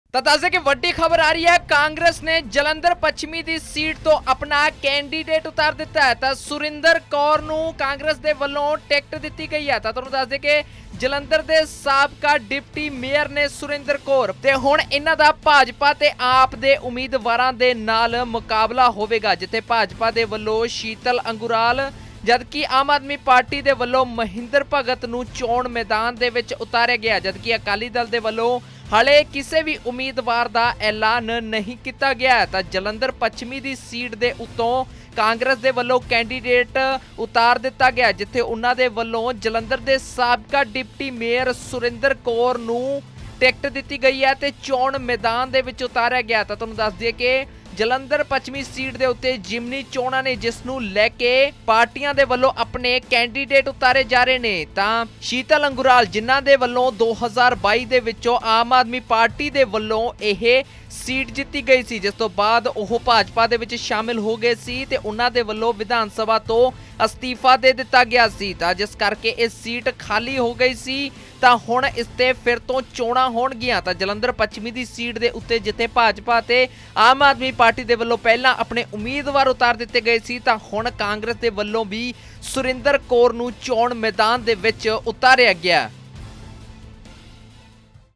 Jalandhar West Congress Candidate|ਕਾਂਗਰਸ ਨੇ ਜਲੰਧਰ ਪੱਛਮੀ ਸੀਟ 'ਤੇ ਉਤਾਰਿਆ ਉਮੀਦਵਾਰ | Surinder Kaur
Jalandhar West Candidate|ਕਾਂਗਰਸ ਨੇ ਜਲੰਧਰ ਪੱਛਮੀ ਸੀਟ 'ਤੇ ਉਤਾਰਿਆ ਉਮੀਦਵਾਰ | Surinder Kaur | Bypoll |N18V